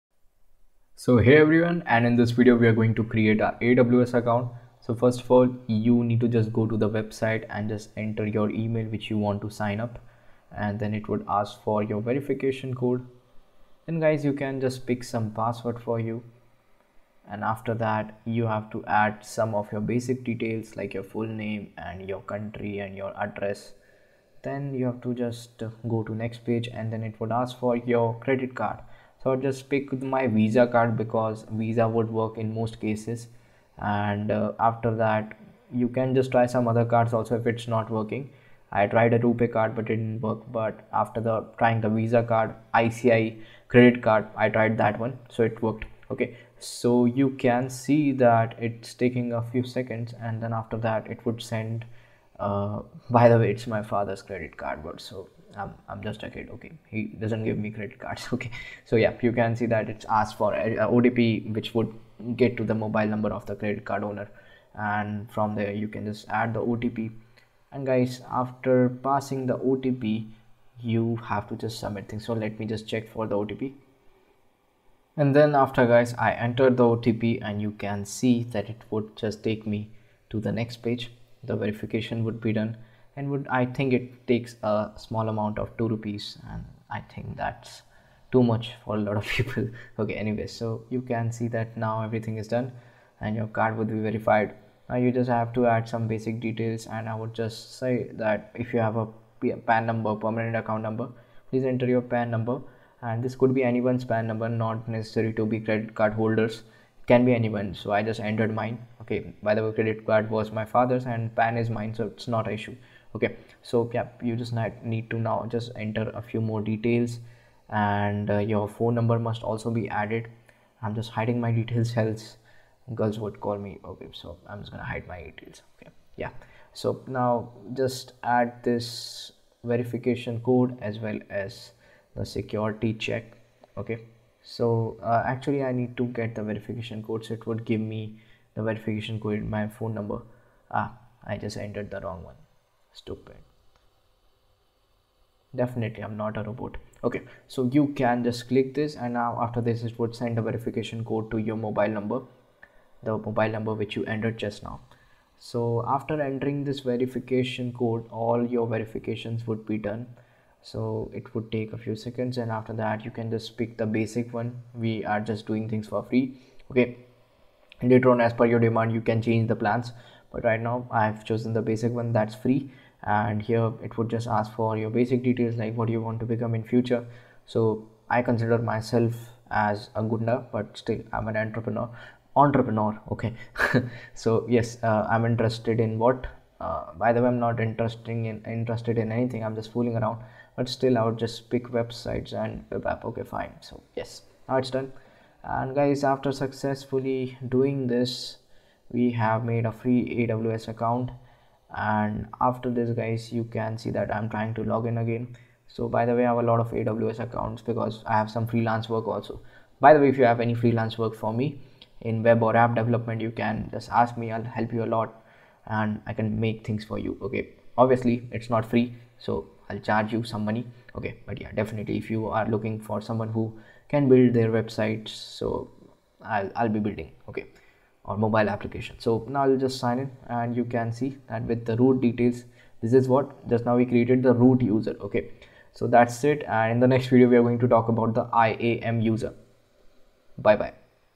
Step-by-Step Guide to Creating a Free AWS Account
🚀 Ready to dive into the world of AWS (Amazon Web Services) but not sure where to start? Look no further! In this comprehensive step-by-step guide, we walk you through the process of creating your very own free AWS account. Whether you're a beginner or have some experience with cloud computing, this tutorial is designed to help you get up and running quickly and effortlessly.

AWS offers a vast array of cloud services, from computing power to storage solutions and beyond, making it an essential tool for businesses and individuals alike. By following along with our easy-to-follow instructions, you'll gain access to the AWS Management Console and unlock the power of the cloud.

In this tutorial, we cover everything you need to know, including:
🔎 How to navigate the AWS website and locate the sign-up page
📝 Step-by-step instructions for creating your AWS account
💡 Tips for choosing the right account type and region
💳 Guidance on setting up billing and payment options
🔒 Best practices for securing your account and managing access

Whether you're looking to deploy your first virtual server, experiment with machine learning algorithms, or build a scalable web application, having an AWS account is the first step on your journey. And the best part? It won't cost you a dime to get started!

So what are you waiting for? Join us as we demystify the process of creating a free AWS account and unlock the full potential of cloud computing. Don't let technical barriers hold you back – with our step-by-step guide, you'll be up and running on AWS in no time!